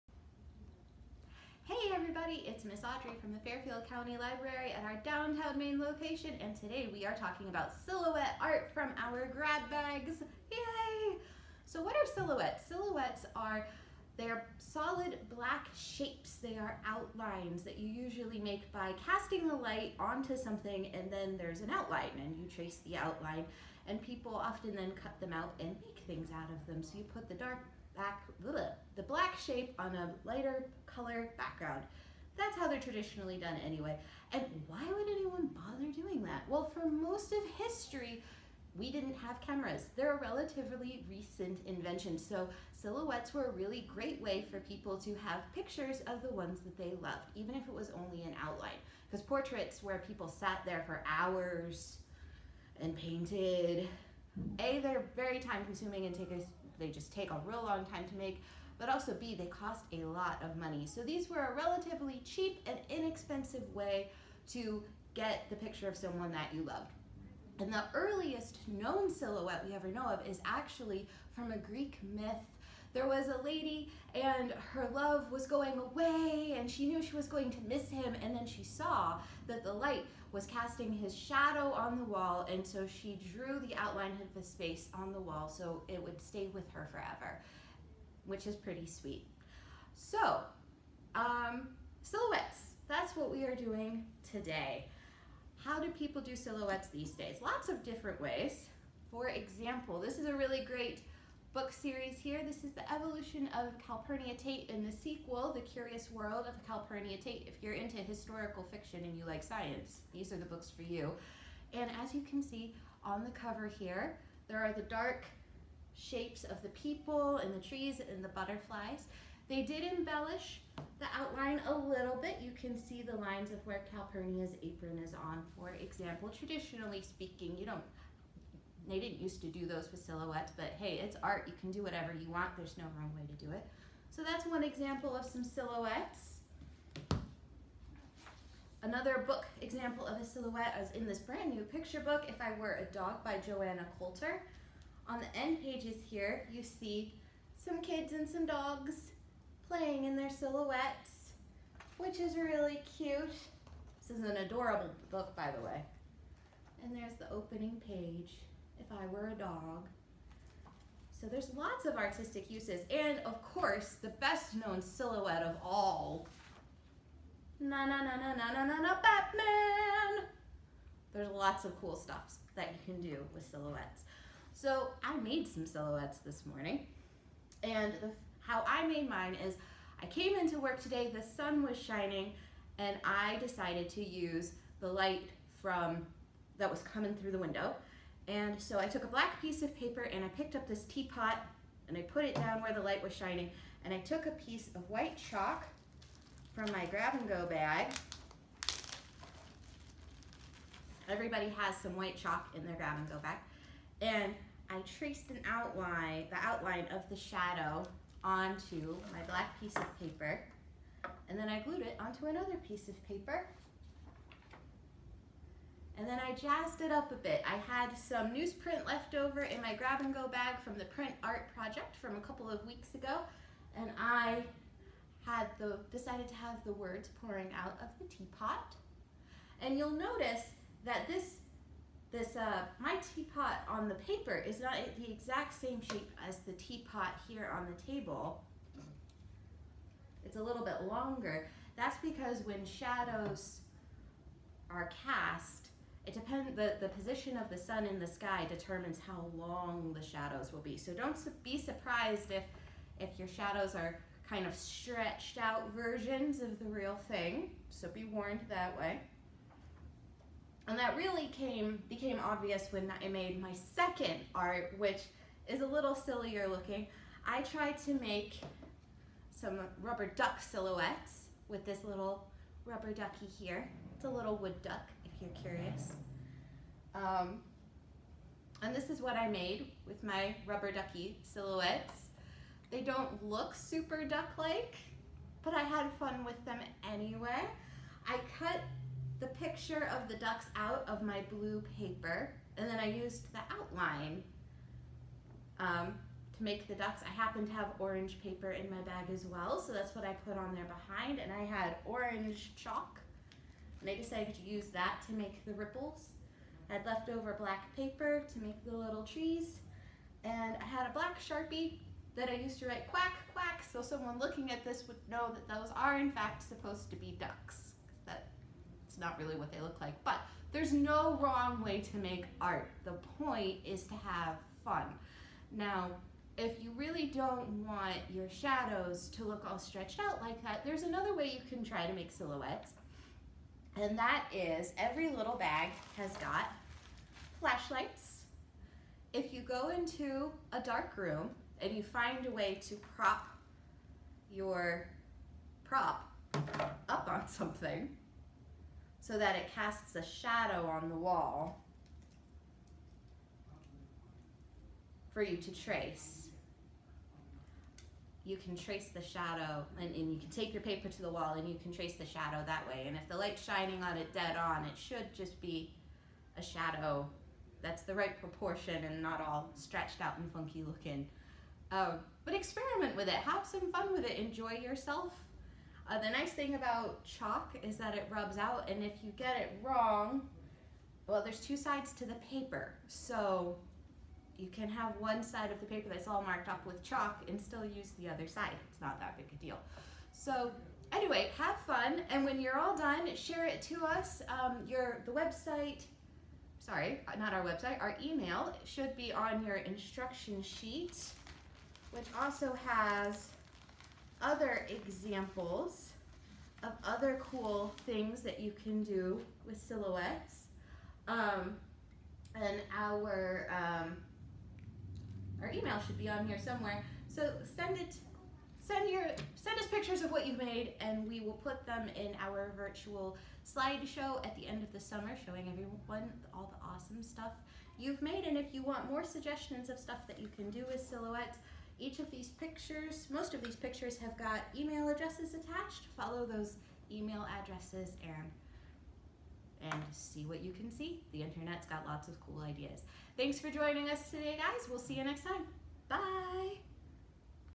Grab Bag Art: Silhouettes
Grab and Go Art projects for kids ages 6-12.
This week we're making Silhouettes. Use the power of the sun or a flashlight to create shadows--then turn them into pictures!